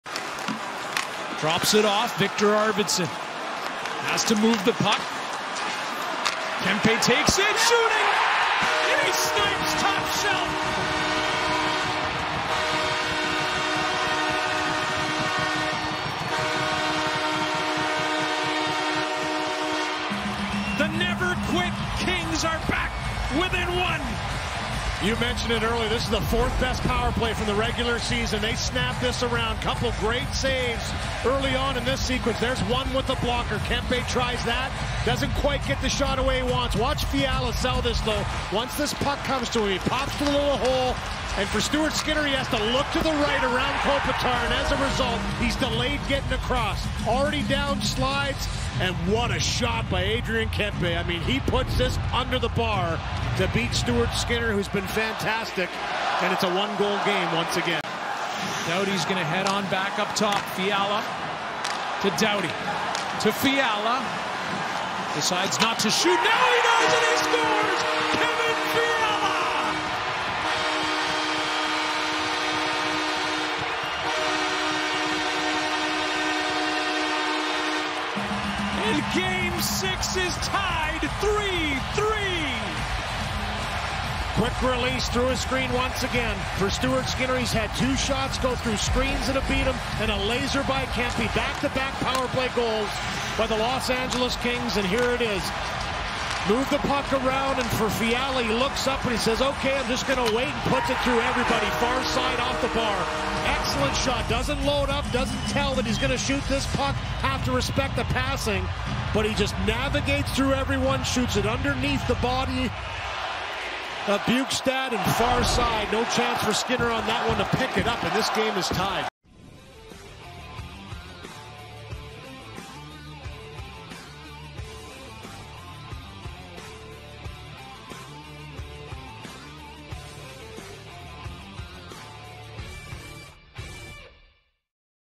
Adrien Kempe And Kevin Fiala Fire Home Back-To-Back Power Play Goals As Kings Tie Game 6 vs. Oilers
Watch as Adrien Kempe and Kevin Fiala score on back-to-back power plays to tie a must-win Game 6 vs. the Edmonton Oilers.